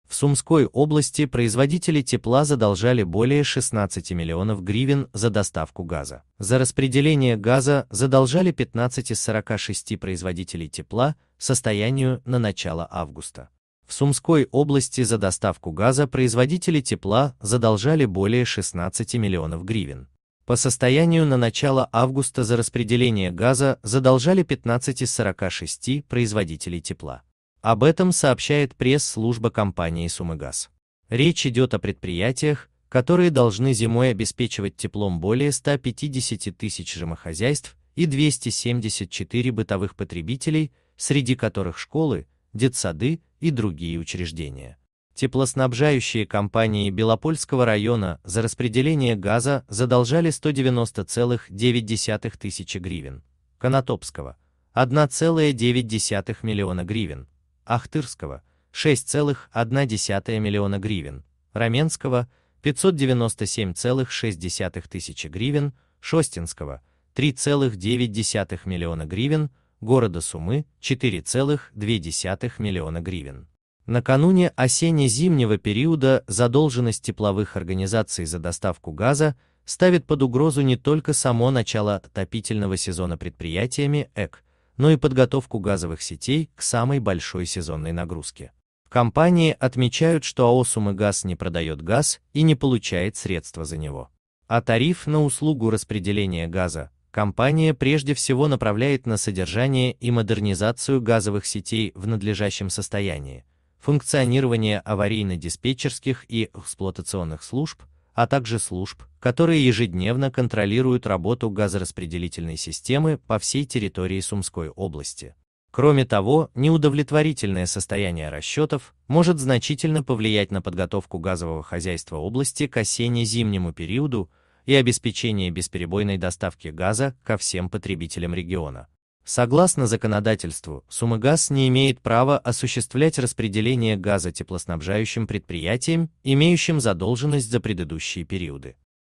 В Сумской области производители тепла задолжали более 16 млн гривен за доставку газа.
За распределение газа задолжали 15 из 46 производителей тепла состоянию на начало августа.В Сумской области за доставку газа производители тепла задолжали более 16 млн гривен. По состоянию на начало августа за распределение газа задолжали 15 из 46 производителей тепла.Об этом сообщает пресс-служба компании "Сумыгаз".Речь идет о предприятиях, которые должны зимой обеспечивать теплом более 150 тыс. жомохозяйств и 274 бытовых потребителей, среди которых школы, детсады и другие учреждения... Источник